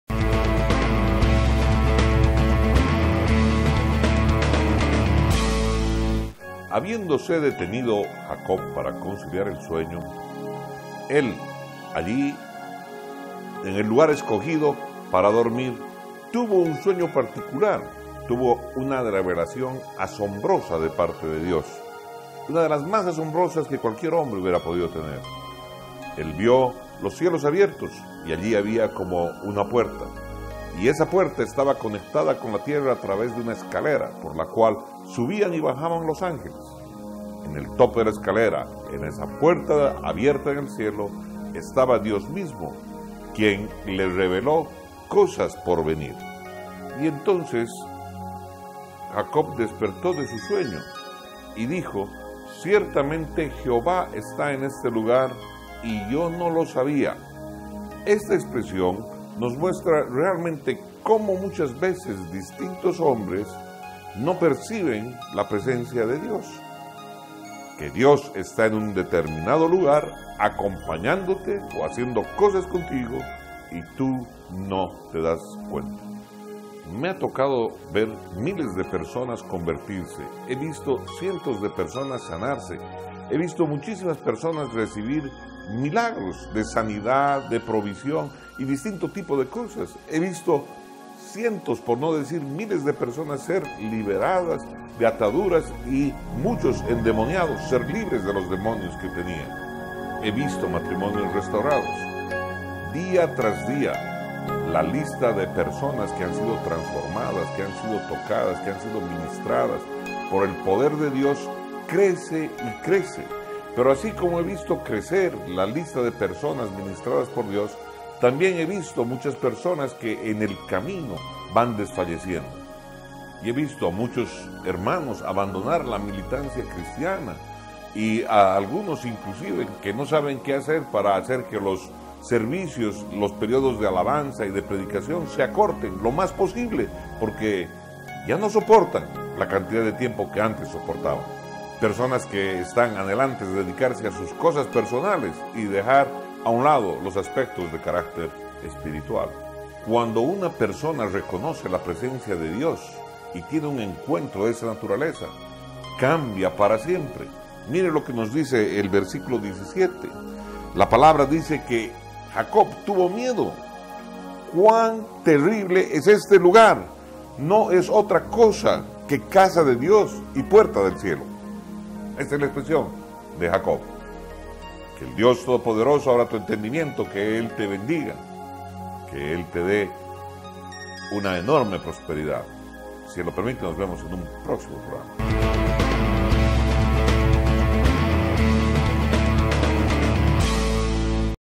Percibiendo La Presencia De Dios - 1x3 - Rafael Vargas Salgueiro
Cuando una persona reconoce la presencia de Dios cambia para siempre.